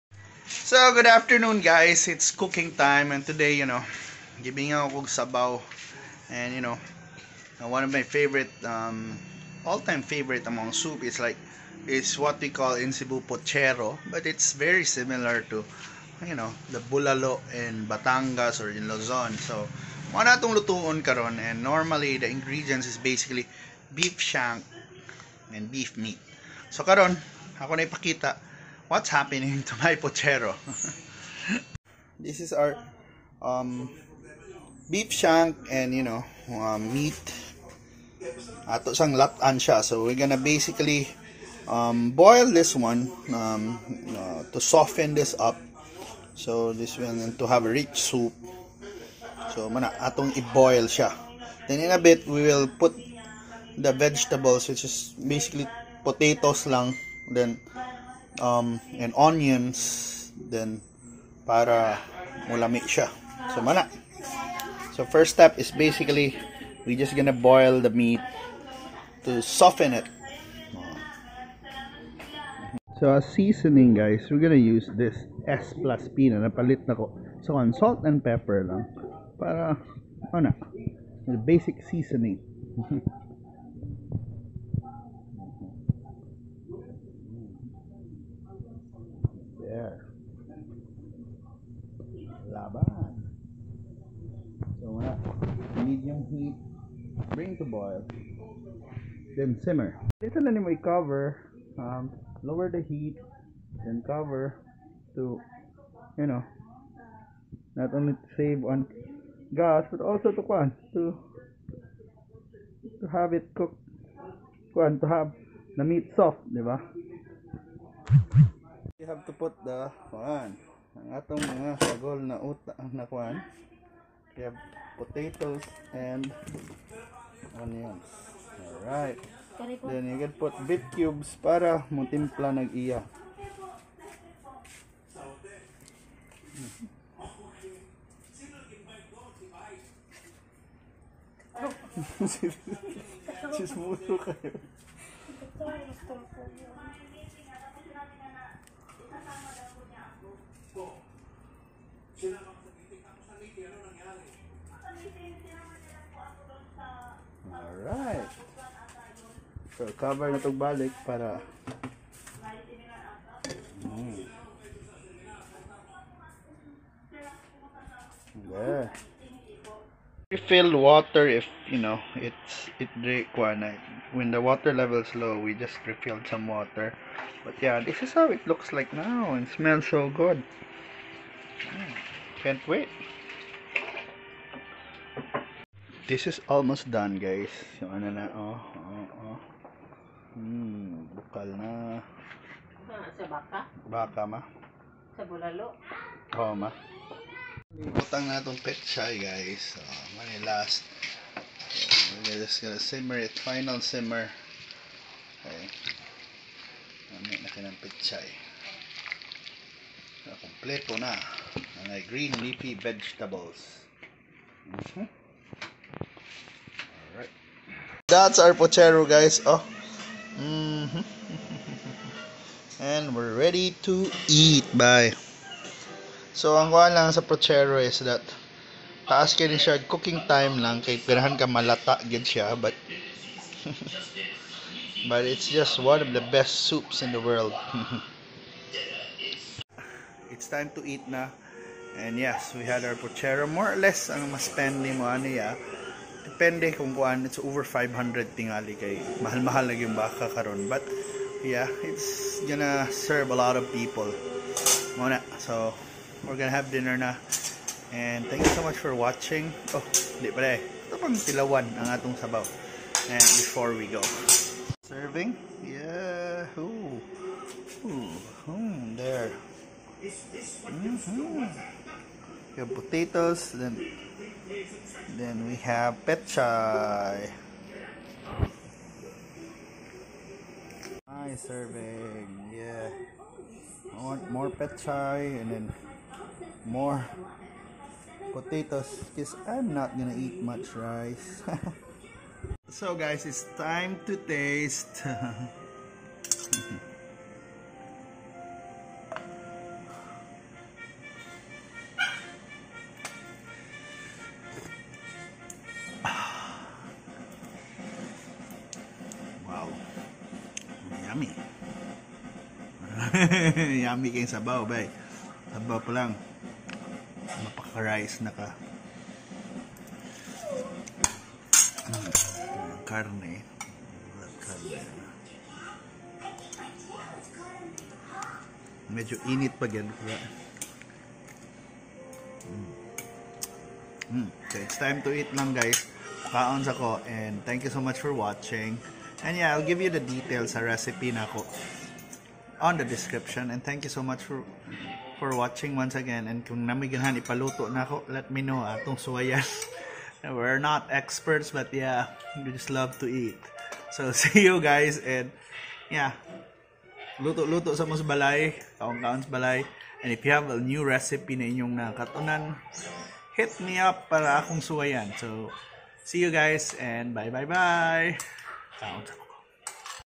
How to Cook a Simple Pochero (Cebuano Beef Shank Soup)
I love soup and one of my favorites is Cebu’s Pochero which is really similar to bulalo.  I like it because the soup is so delicious and rich.  Before, I love sucking on the bone marrow.   The tendons are also so enjoyable to eat.  Plus it’s so easy to cook this dish.

For this particular day, here are my ingredients used:

1.  Beef shank
2.  Beef ( Rustan’s Ayala have what they call Pochero Cut)
3.  Water
4. Salt and Pepper
5.  Beef Cubes
6. Potatoes
7. Onions
8. Penchant

You can also add items like Corn and some even use heart of palm.